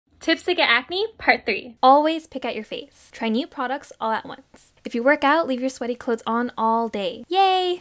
Tips to Get Acne #3 | FaceTory
The best way to get acne! 😉
P.S. Yes this is a joke! We are showing you what NOT to do to your skin!!

Shop our favorite products for acne!
✨ FaceTory Oats Calming Glow Weightless Facial Oil
Have more skincare questions?